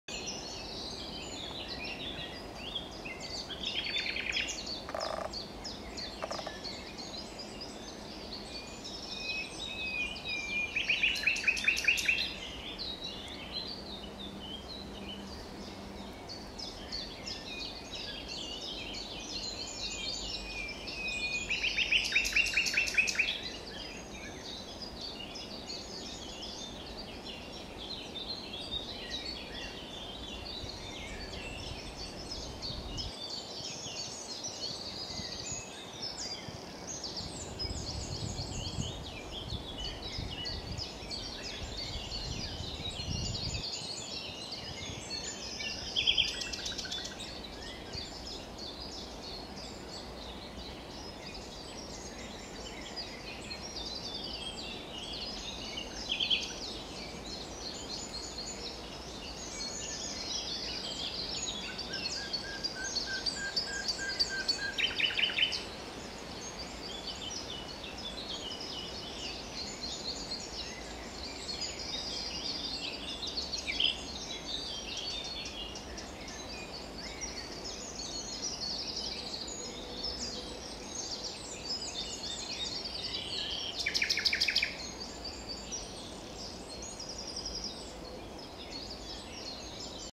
Common Nightingale
Common Nightingale singing it's song, Wijgmaalbroek, 22/04/2020